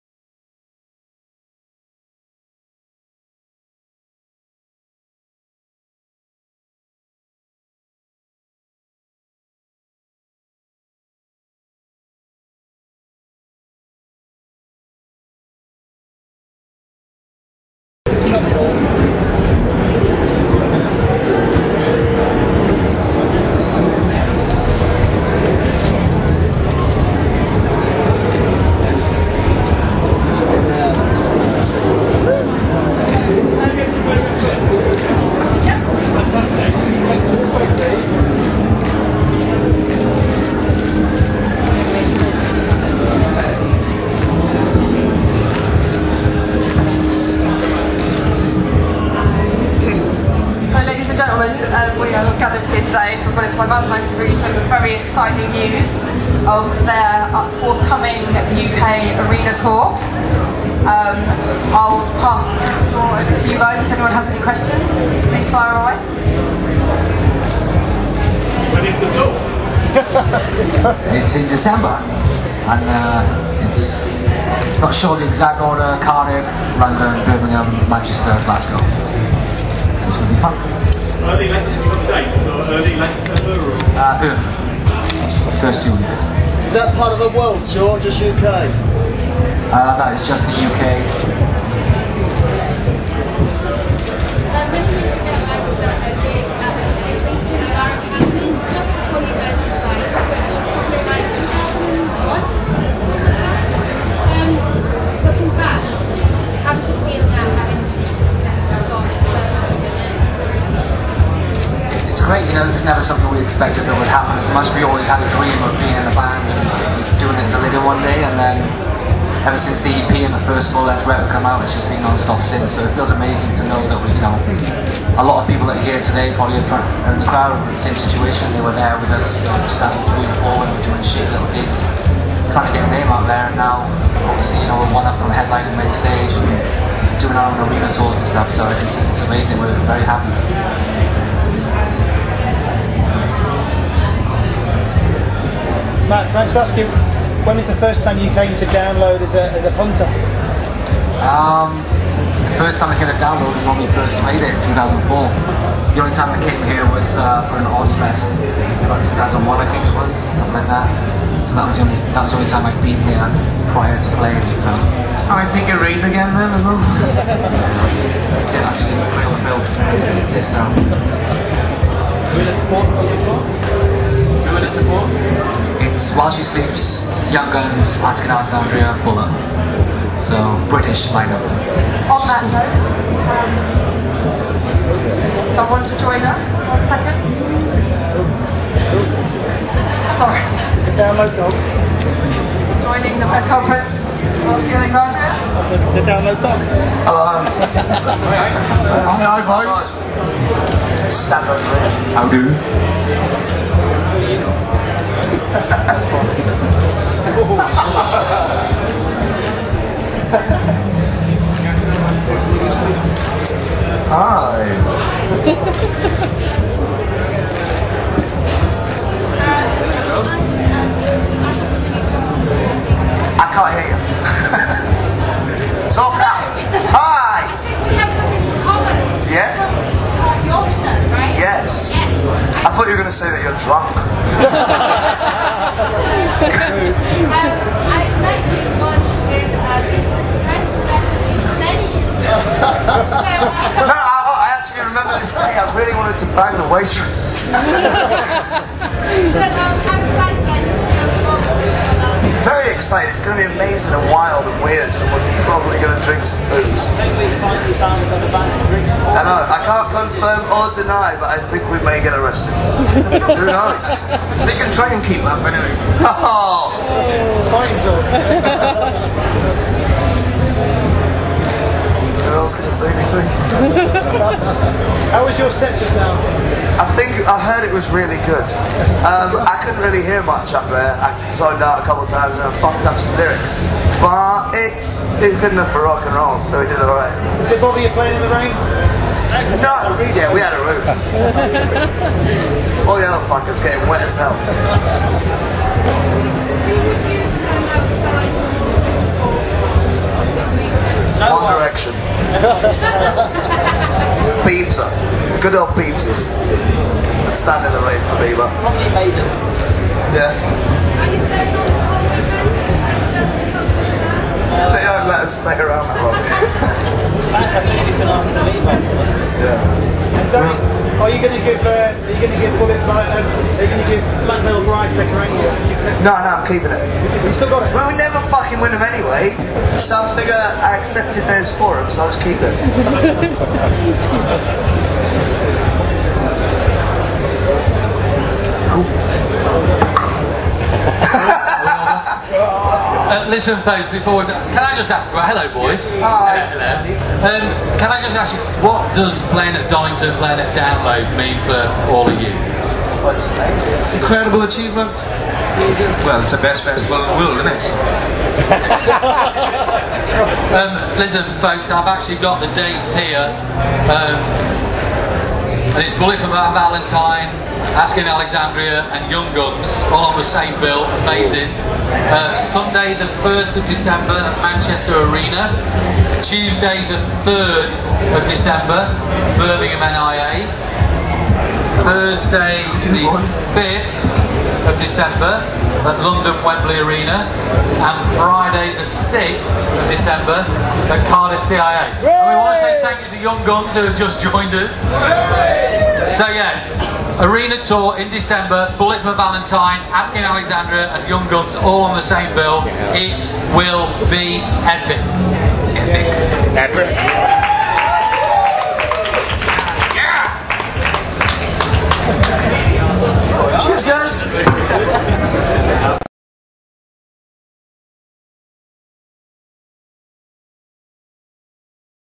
BULLET FOR MY VALENTINE Download Press Conference
BFMV announce UK tour with ASKING ALEXANDRIA and YOUNG GUNS at Download 2013